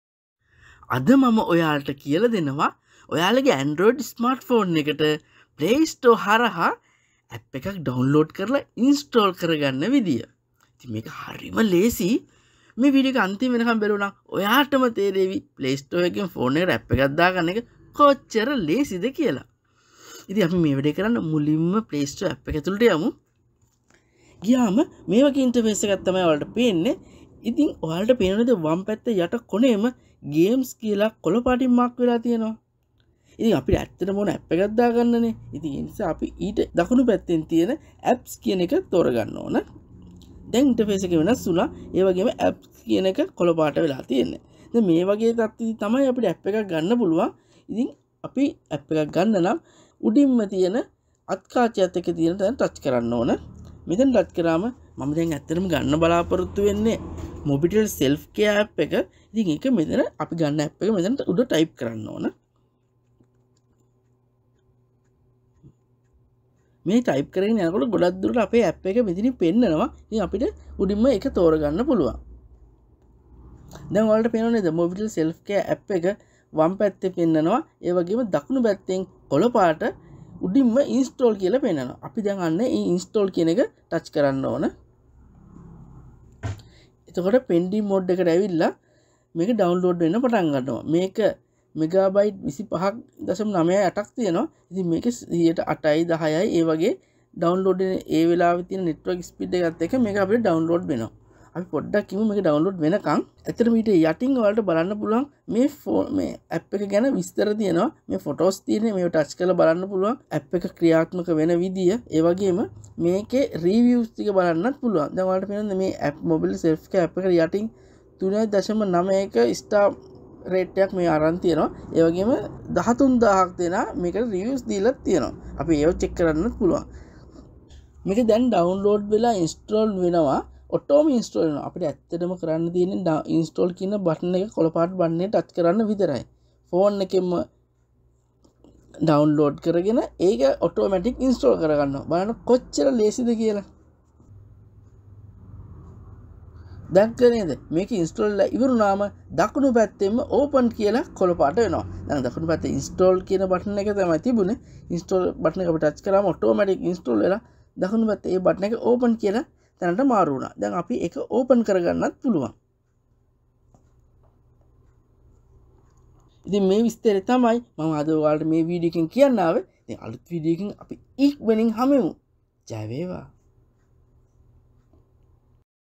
45 - ෆෝන් එකට ඇප් එකක් ගන්නෙ මෙහෙමයි | How to get app to my phone
අපගේ ඇන්ඩ්රොයිඩ් දුරකථනය හරහා වැඩ කටයුතු පහසු කර ගැනීමට සහාය දක්වන විවිධ ඇප් වර්ග මේ වන විට නිකුත්වී අවසන්.
මෙම විවිධ වූ යෙදුම් අතරින් අපගේ කාර්යයන් පහසු කර ගැනීමට අපගේ දුරකථනයට අපට අවශ්‍ය වන ඕනෑම ඇප් එකක් වඩාත් ලේසියෙන් හා නිවැරදිව, ඉක්මනින් ලබා ගැනීම ගැන මෙම වීඩියෝවෙන් විස්තර කෙරෙනවා.